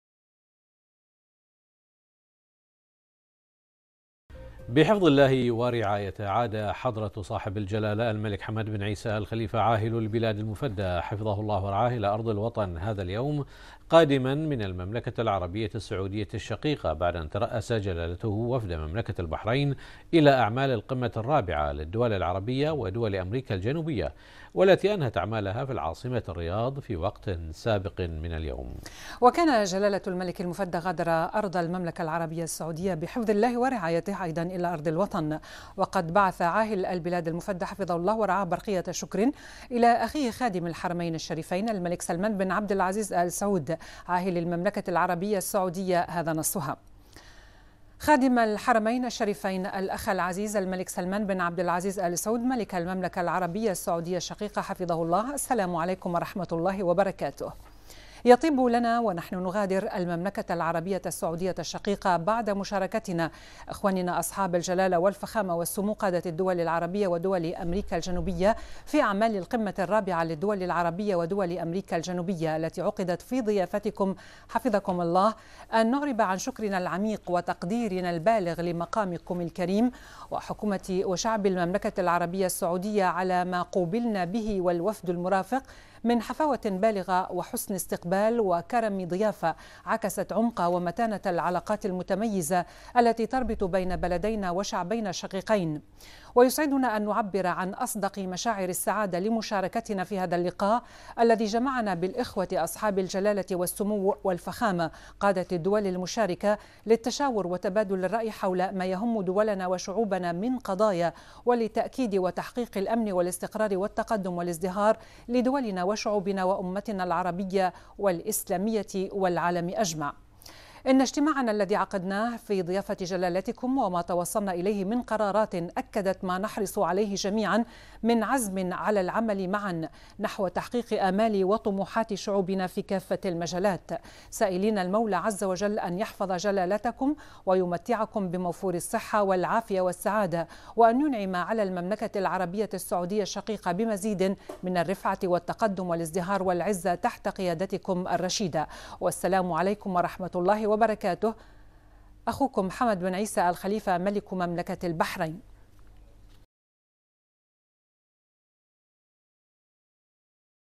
البحرين : عاهل البلاد يعود الى ارض الوطن قادما من السعودية
الوصف

عاهل البلاد يعود الى ارض الوطن قادما من السعودية